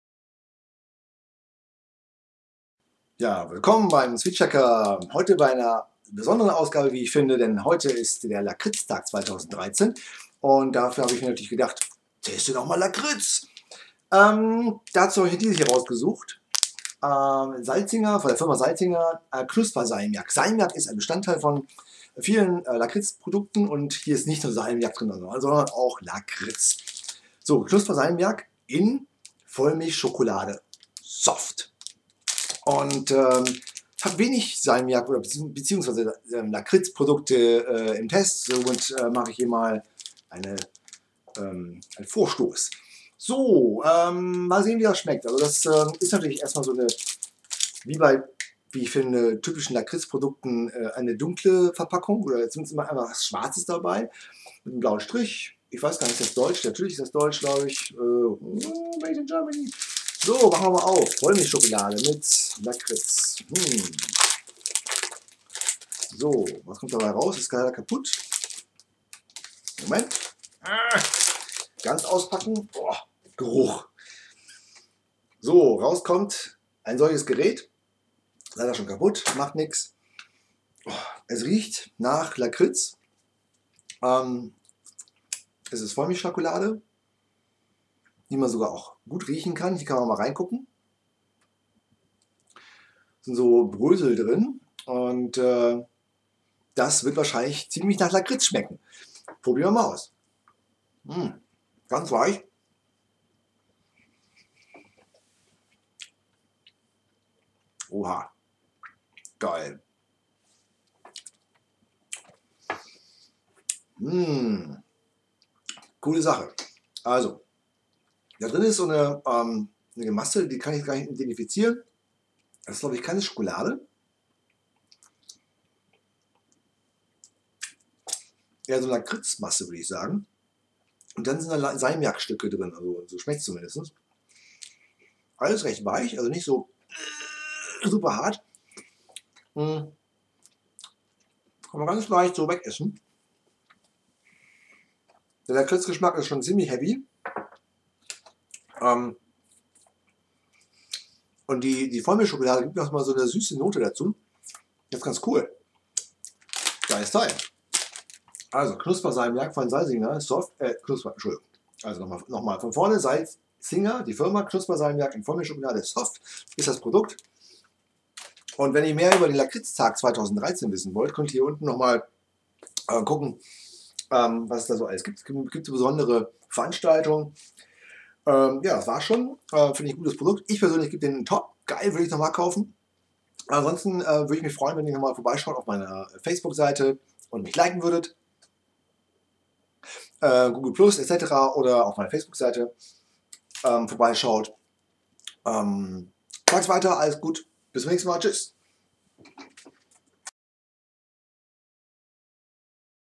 SweetsChecker -Ausgabe #84: Dunkler Riegel für den Lakritztag - Candy Review
Diese Ausgabe wird vom Lakritztag bestimmt. Heute ist Lakritztag 2013, der erste Lakritztag überhaupt der in der ganzen Nation gefeiert und zelebriert wird. Hurra. In dieser Ausgabe dreht es sich um Knusper-Salmiak in Vollmilschschokolade soft der Firma: Salzinger. Das hört sich ja schon toll an: Es ist Knusprig, mit Schokolade, weich soll es sein und es ist Salmiak enthalten (und Lakritz natürlich!). Wie das wohl schmeckt, könnt ihr in meinem Video sehen.

Wenn dir diese Ausgabe gefallen hat, würde ich mich über ein like und einem Share/ReTweet an deine Freunde freuen. Besten Dank.